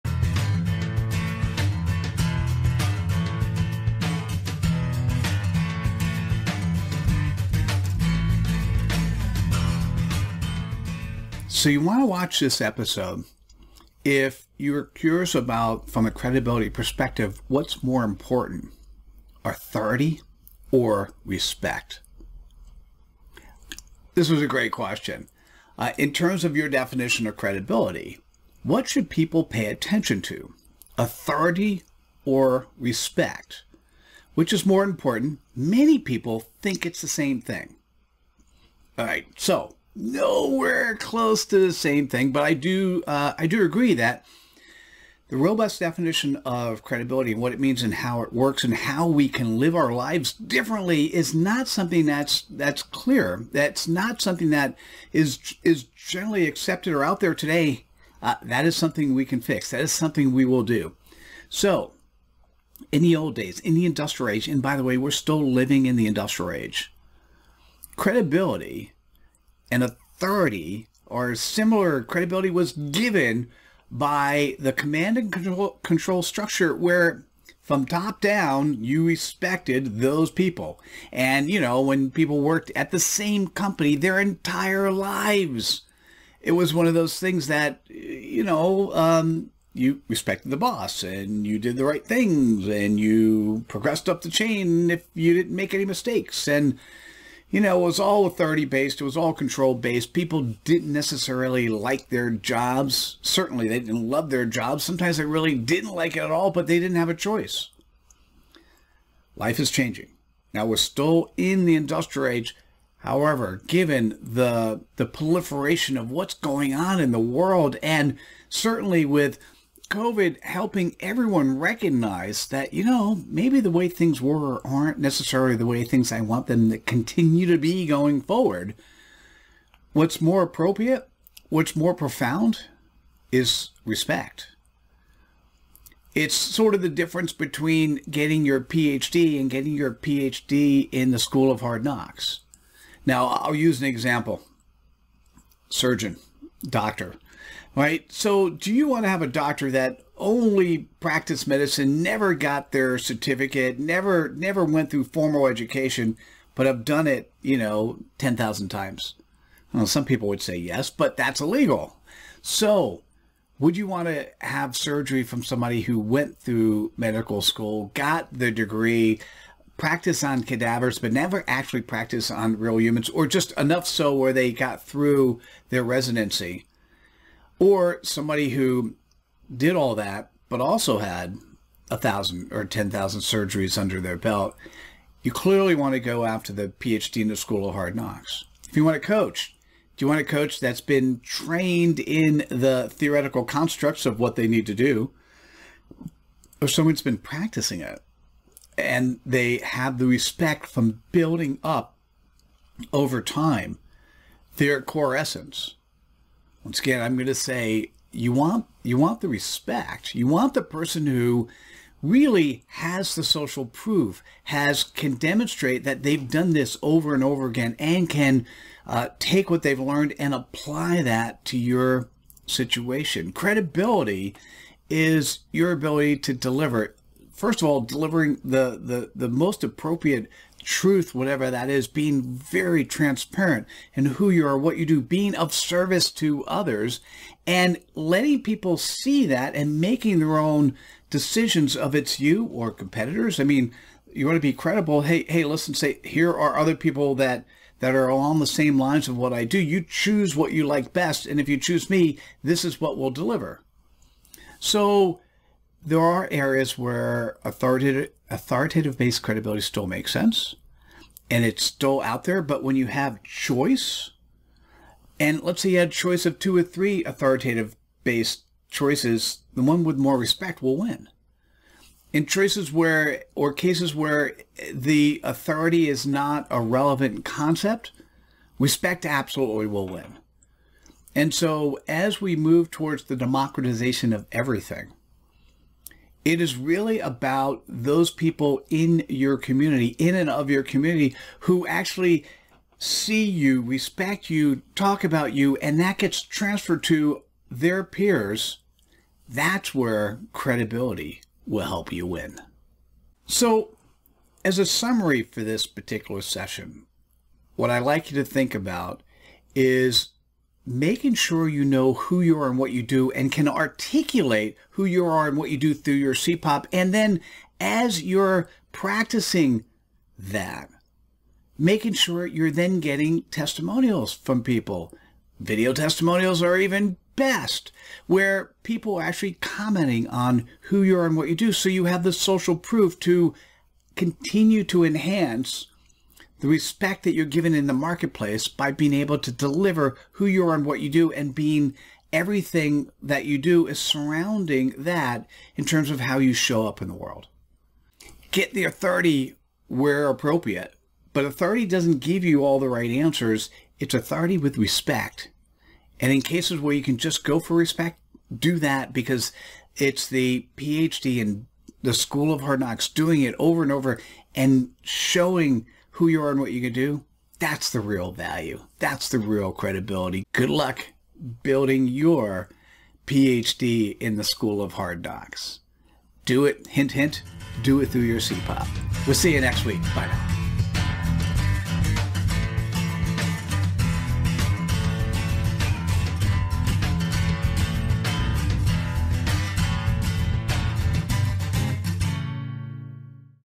Authority vs Respect (DCE 017)
In this episode, Mitchell Levy sheds light on a confounding question, “which is more important, authority or respect?” What do you think is the answer?

This week, Mitchell piques our curiosity as he delves deep into why having a Ph.D. in the School of Hard Knocks is more paramount than just procuring a title through pedagogies and assiduously learning the theories and technical know-how in the four corners of a study hall.

How do you think those relate to credibility?

Here are highlights from this episode:

→ Most people think authority and respect are the same thing, however, it isn’t necessarily the case. Authority used to be control-based; but authority with respect is vouched for by outcome, SocialProof, and empirical evidence in the marketplace.

→ As people seek Expertise, they don’t merely have proclivity towards pundits with Ph.D. who are trained in the sphere of theoretical constructs, they unequivocally have a special predilection for those who obtained theirs in the School of Hard Knocks!

→ Authority doesn’t give you all the right answers, it is authority with Respect!

Let us know your thoughts about this video in the comments. Looking forward to your responses.

Credibility Nation is a membership community where individuals and corporations learn how to do business in a credible way.